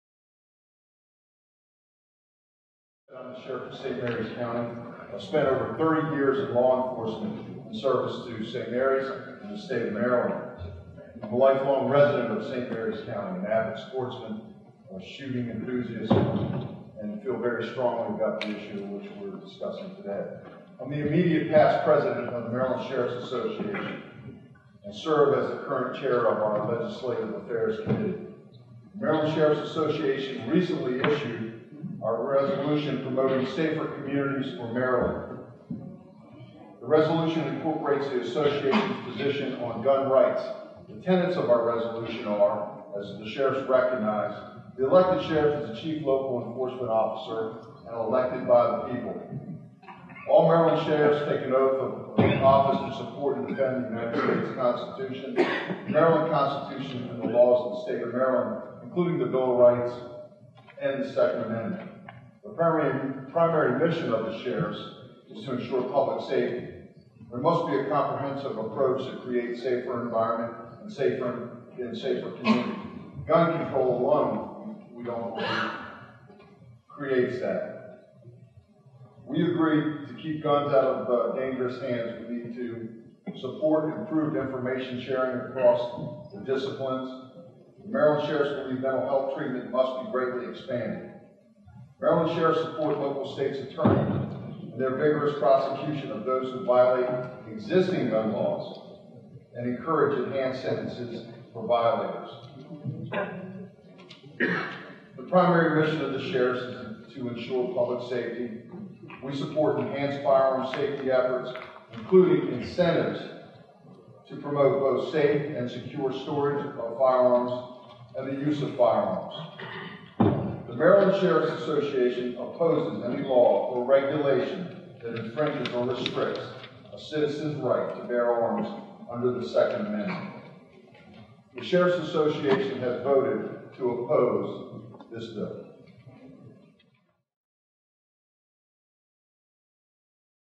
Sheriffs Association is against new gun control
A record breaking number of Marylanders flooded Annapolis to protest senate passage of Governor O'Malley's new gun control.
Then the House committee hearing lasted until a record breaking 3:45 am Saturday morning, with 97.6 percent of all testimony against the proposed gun ban.
Now is the critical time for every Maryland gun owner to visit their representatives local office and tell them they oppose the ban of commonly owned pistols and rifles.
Tell them that House Bill 294 is a de facto total gun ban because it seeks to drive every gun dealer and manufacturer out of the state by threatening 3 years in prison for a paperwork mistake.
Tell them that House Bill 294 will cost millions in lost tax revenues at a time the governor wants to raise gas taxes.
Tell them it seeks to ban the most common competitive pistols and rifles to include those used in the Olympic games, at a time Baltimore may attempt to secure a bid for the 2024 Olympics.
Sometimes politicians are just ignorant.
Visit your representatives today and educate them,  because it's no one else's responsibility but yours.